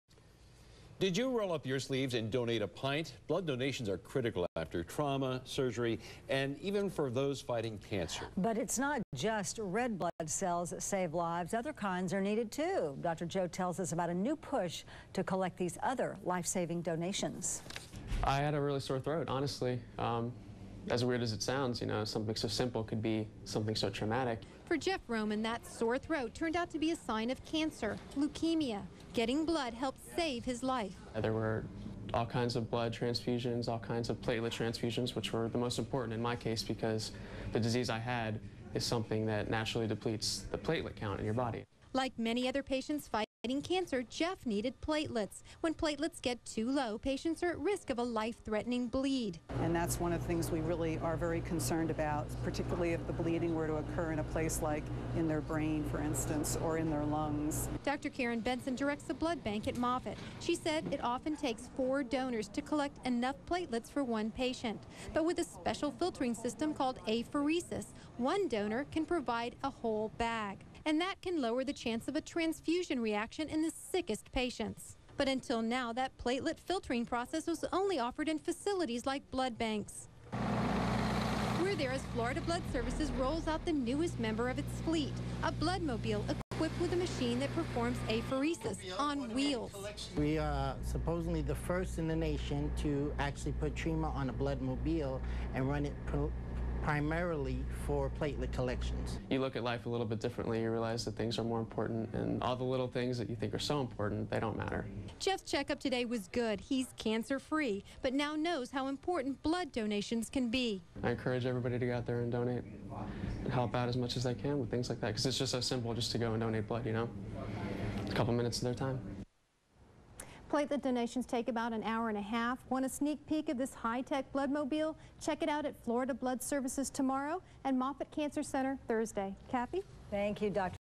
New Apheresis Bloodmobile
See our newest bloodmobile - fully equipped for apheresis collection.